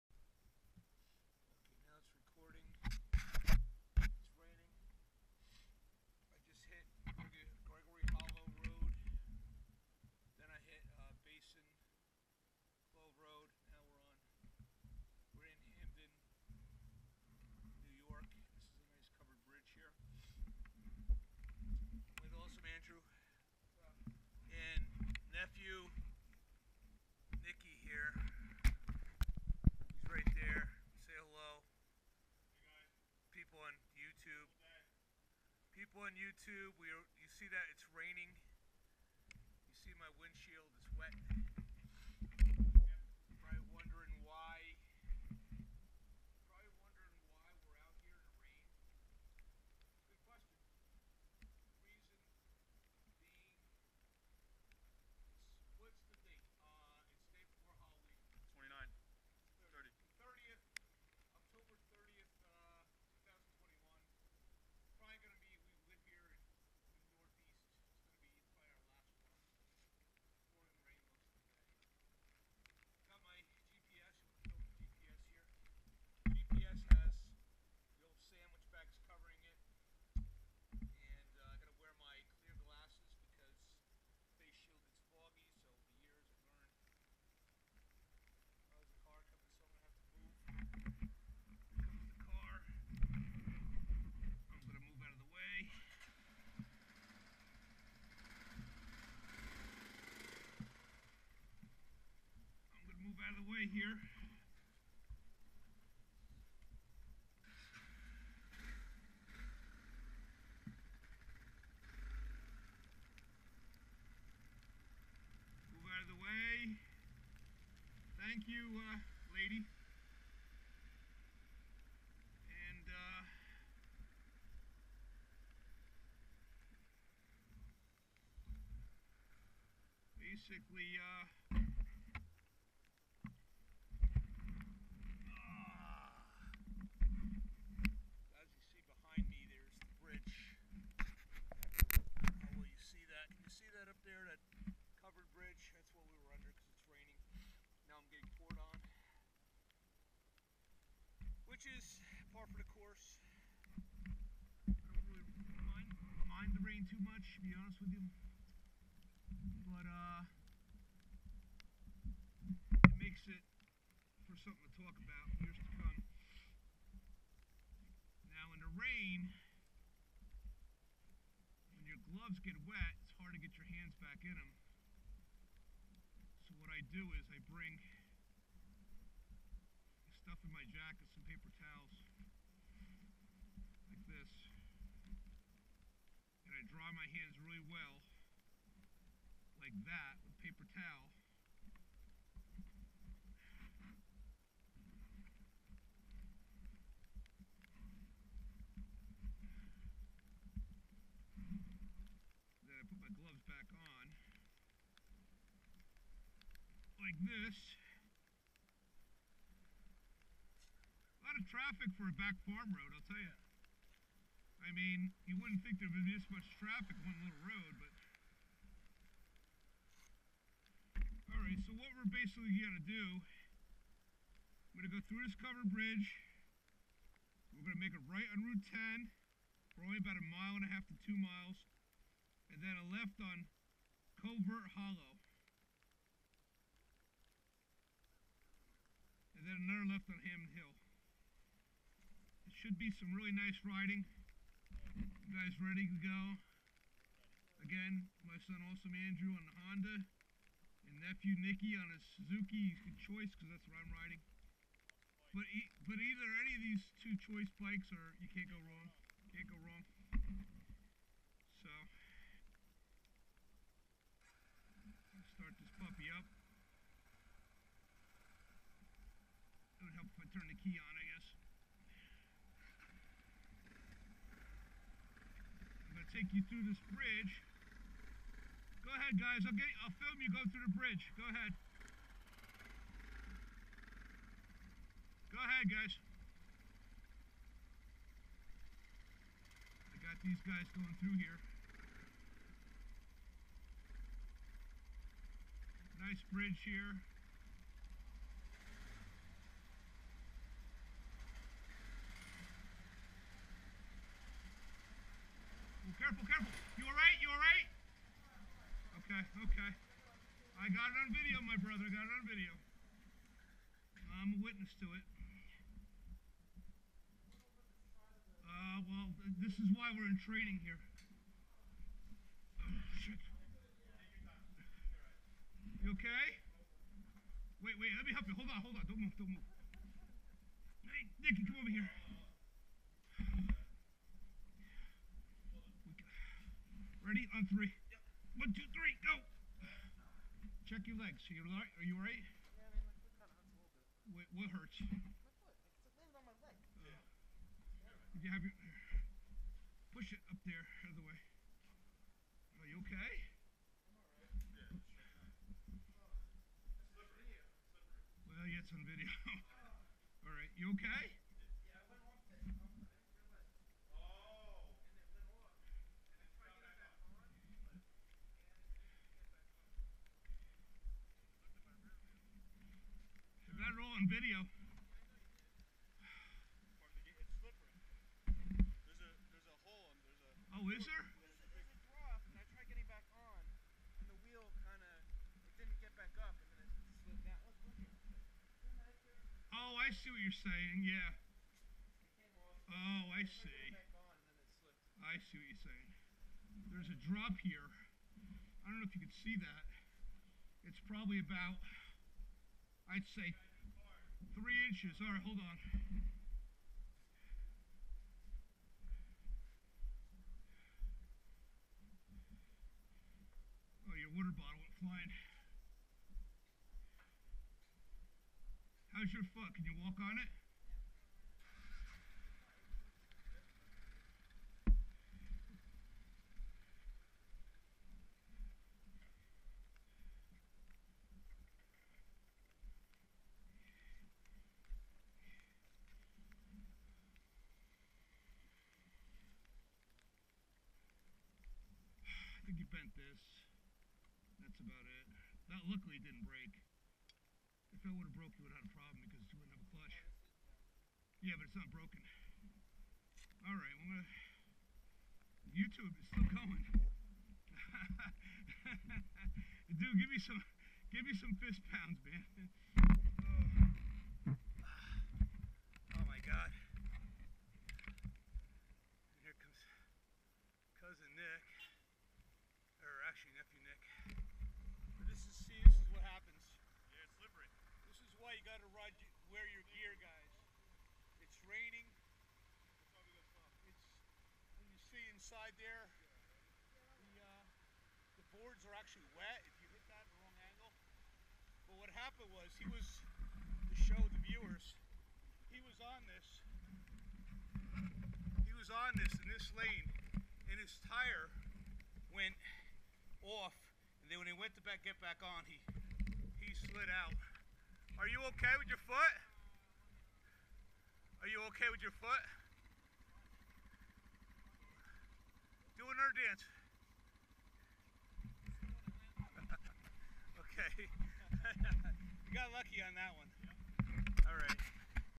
New York State Dual Sport Covered Bridge Crash Hamden NY
My son Awesome Andrew riding his 2015 Honda XR650L through Hamden Covered Bridge in Hamden NY on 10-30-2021. He rode off the main section to a 3 inch lower section of bridge and when he tried to go back his wet tires washed out on the angle. He was ok not hurt. The rear luggage supports helped save his leg from getting crushed by the bike